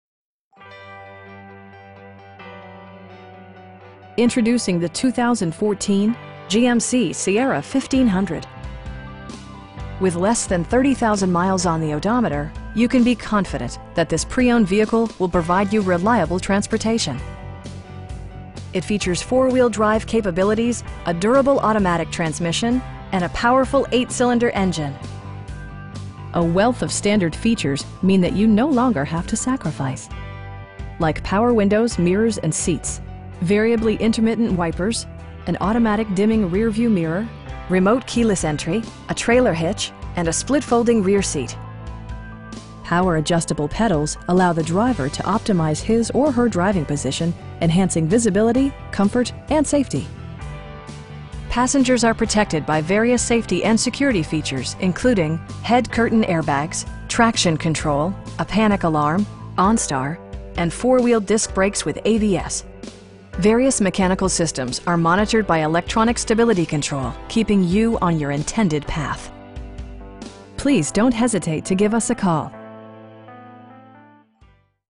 2014 GMC Sierra 1500 SLT in Montgomery, AL 36117
Brewbaker Preowned
300 Eastern Blvd.

Introducing the 2014 GMC Sierra 1500. With less than 30,000 miles on the odometer, you can be confident that this pre-owned vehicle will provide you reliable transportation. It features four-wheel drive capabilities, a durable automatic transmission, and a powerful 8 cylinder engine. A wealth of standard features means that you no longer have to sacrifice. Like power windows, mirrors, and seats, variably intermittent wipers, a built-in garage door transmitter, an automatic dimming rear-view mirror, tilt and telescoping steering wheel, remote keyless entry, a trailer hitch, and a split folding rear seat. Features such as automatic climate control and leather upholstery prove that economical transportation does not need to be sparsely equipped. Power adjustable pedals allow the driver to optimize his or her driving position, enhancing visibility, comfort and safety. Passengers are protected by various safety and security features, including: dual front impact airbags with occupant sensing airbag, head curtain airbags, traction control, a panic alarm, onStar, and 4 wheel disc brakes with ABS. Various mechanical systems are monitored by electronic stability control, keeping you on your intended path. We pride ourselves in the quality that we offer on all of our vehicles. Please don't hesitate to give us a call.